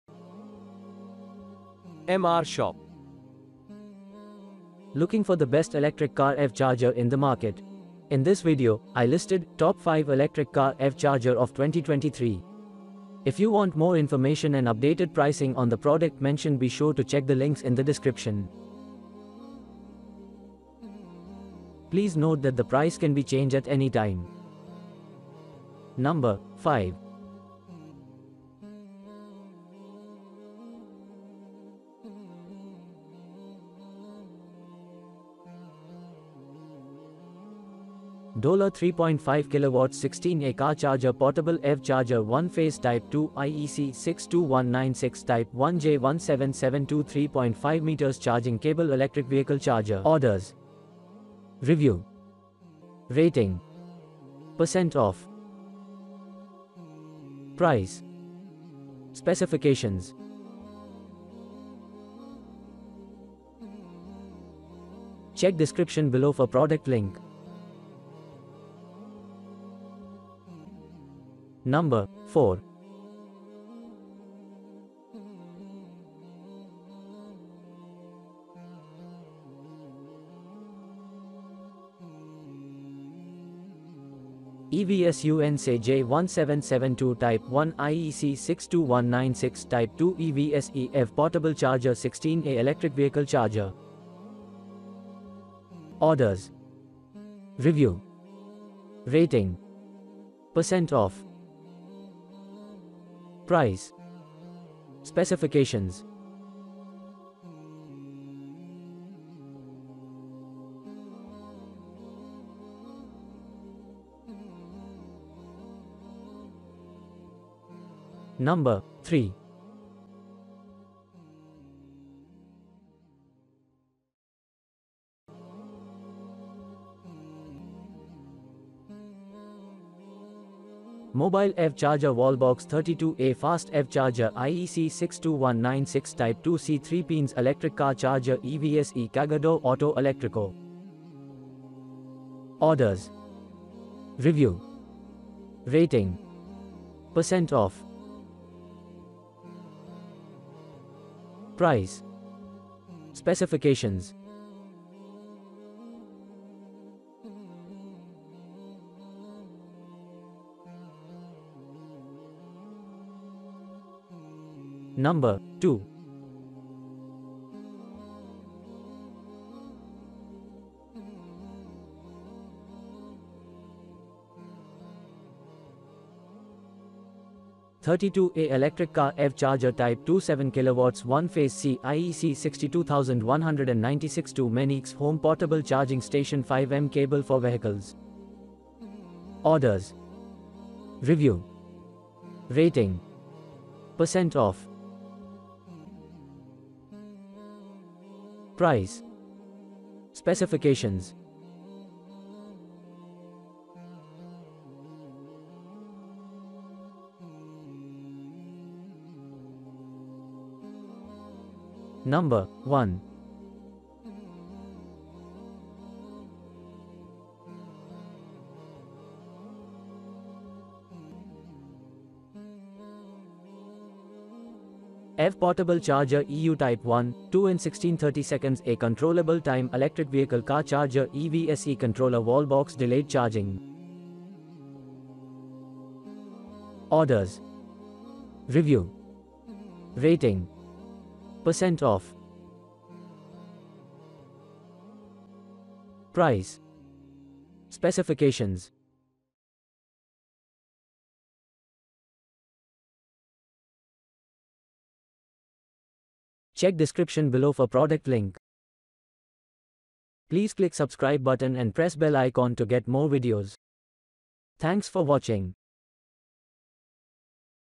Top 5 Best EV Charger In 2023 | Electric Vehicle Charger
05. Daolar 3.5KW 16A car charger Portable ev charger 1phase Type 2 IEC62196 Type1 J1772 3.5m Charging Cable Electric Vehicle Charger

04. EVSUN SAE J1772 Type 1 IEC62196 Type 2 EVSE EV Portable Charger 16A Electric Vehicle Charger

03. Mobile EV Charger Wallbox 32A Fast EV Charger IEC62196 Type 2 CEE 3 Pins Electric Car Charger EVSE Cargador Auto Electrico

02. 32A Electric Car EV Charger Type 2 7KW 1 Phase CEE IEC 62196-2 Mennekes Home Portable Charging Station 5M Cable for Vehicles

01. EV Portable Charger EU Type 1/ 2 16/32A Controllable Time Electric Vehicle Car Charger EVSE Controller Wallbox Delayed Charging